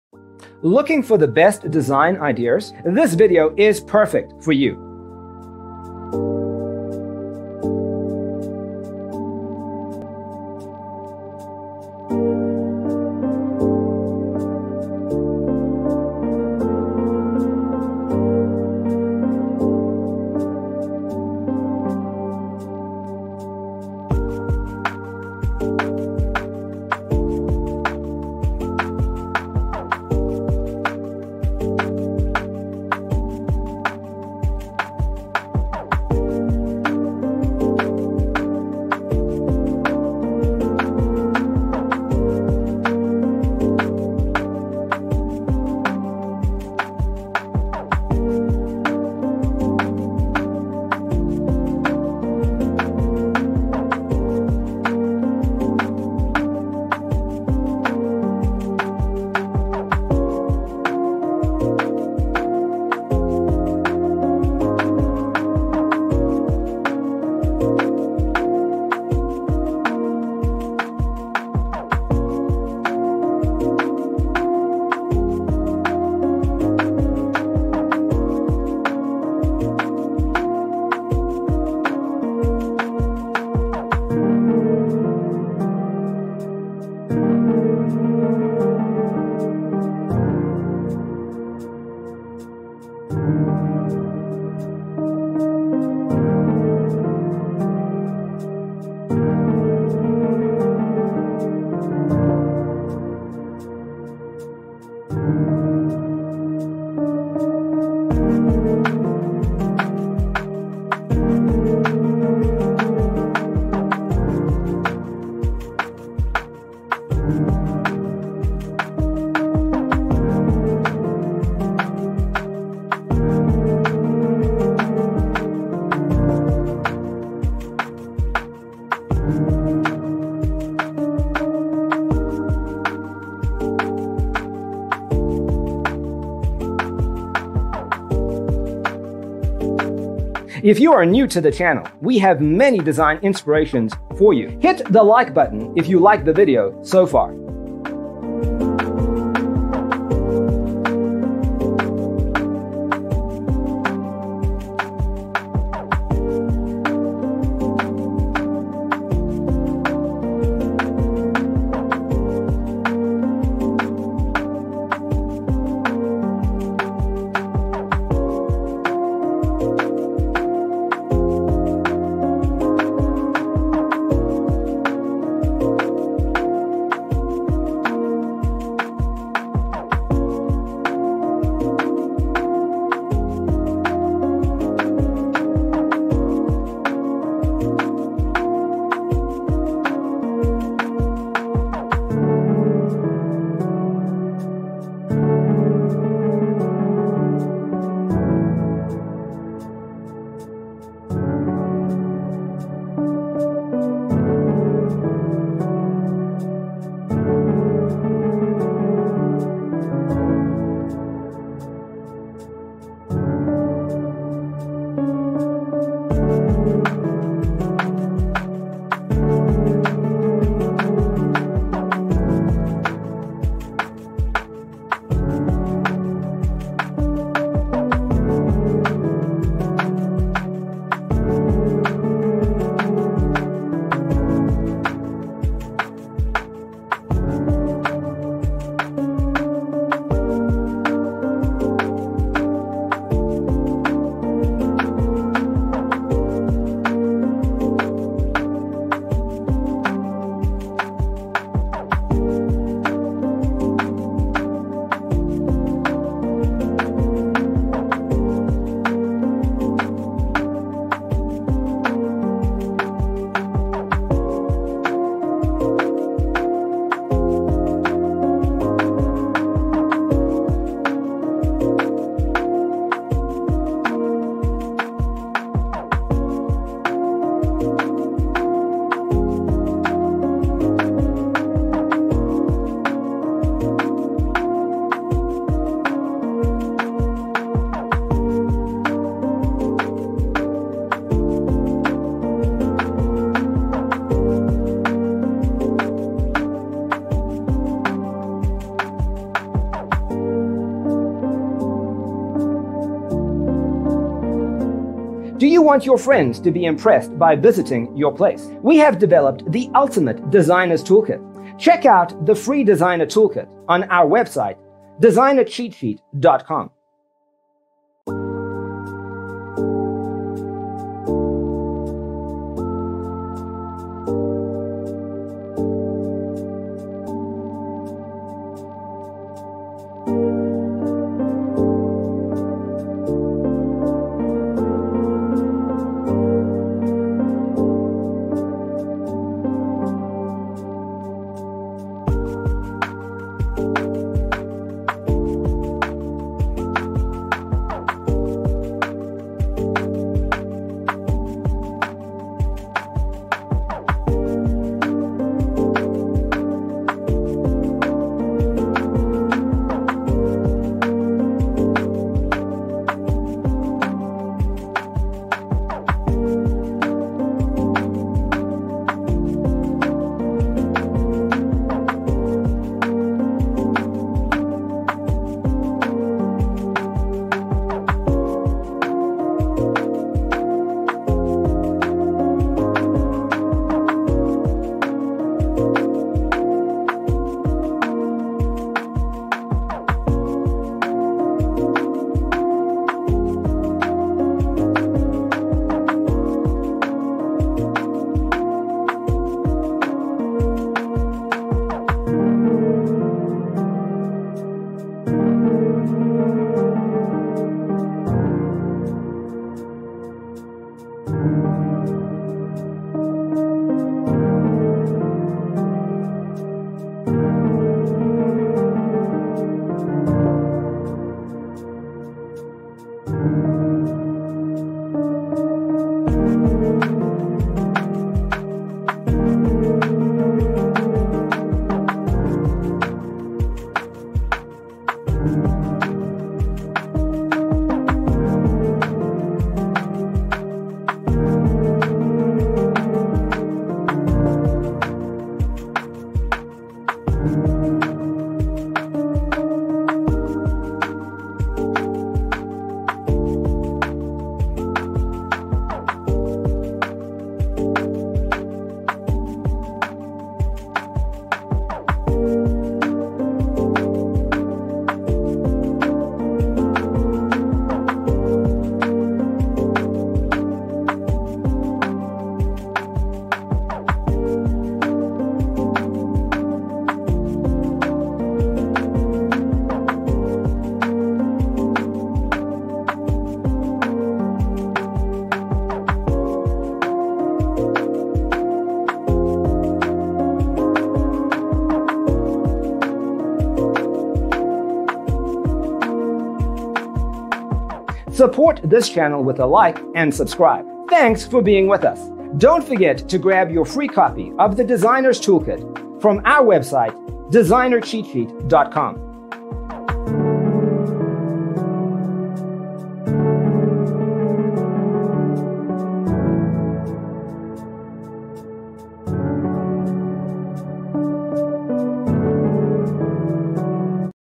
75 Traditional Beige Laundry Room Design Ideas You'll Love ➤
Browse traditional laundry room ideas and decor inspiration. Discover designs for custom traditional laundry rooms and closets, including traditional utility room organization and storage solutions.

0:00:00 - Intro
0:00:12 - Traditional Beige Laundry Room Design Ideas You'll Love: Inspirations 1 to 25
0:03:03 - Traditional Beige Laundry Room Design Ideas You'll Love: Inspirations 26 to 50
0:06:07 - Traditional Beige Laundry Room Design Ideas You'll Love: Inspirations 51 to 75

👋 Well Hello!